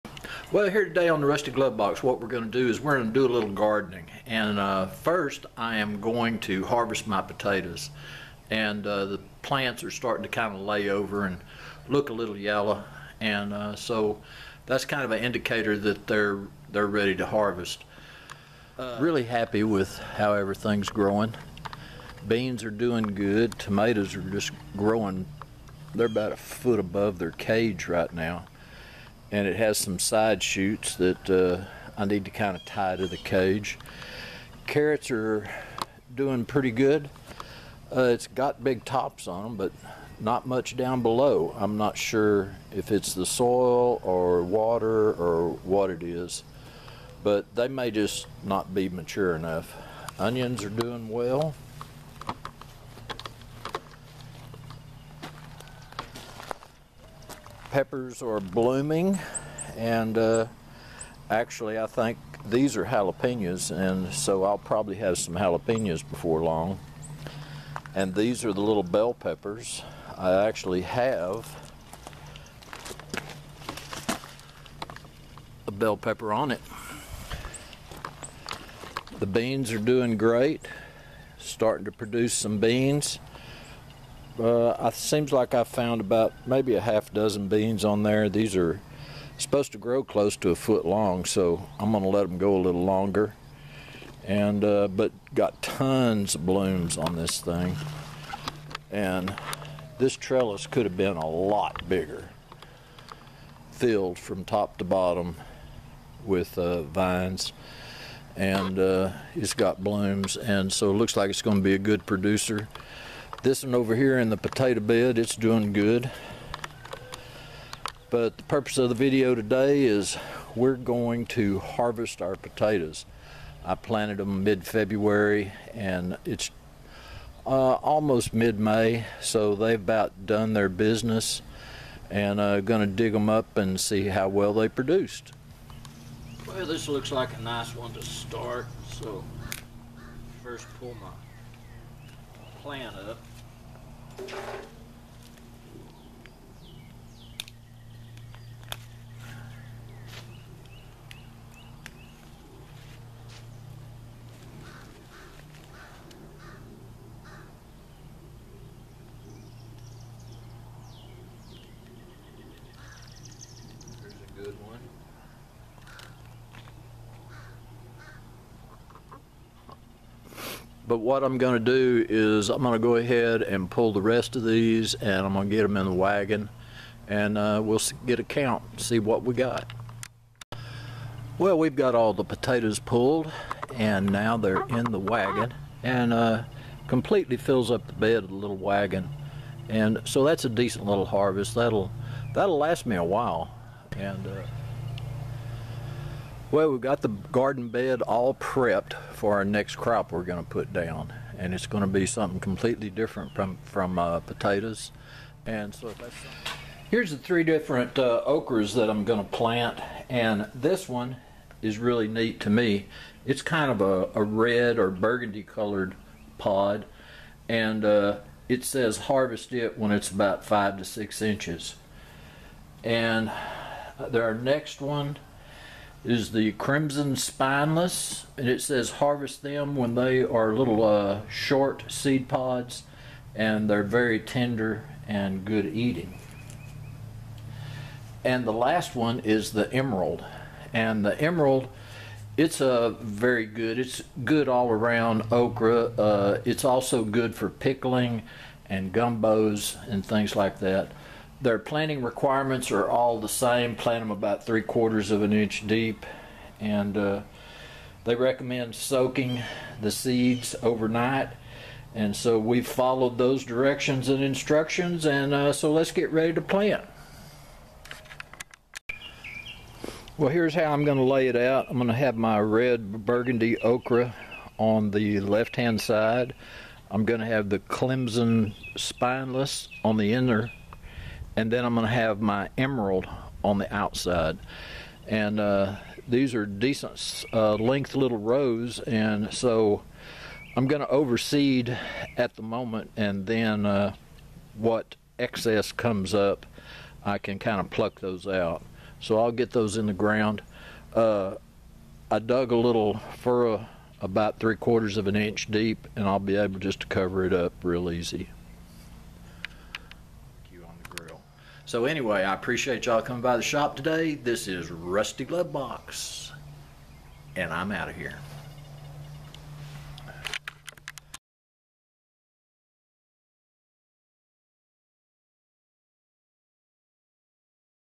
Potato harvest and 3 Okra varieties to be planted.
Had a very productive potato harvest . They will be great on the grill or baked in the oven.  Planting 3 okra varieties in their place and looking forward to see how they do.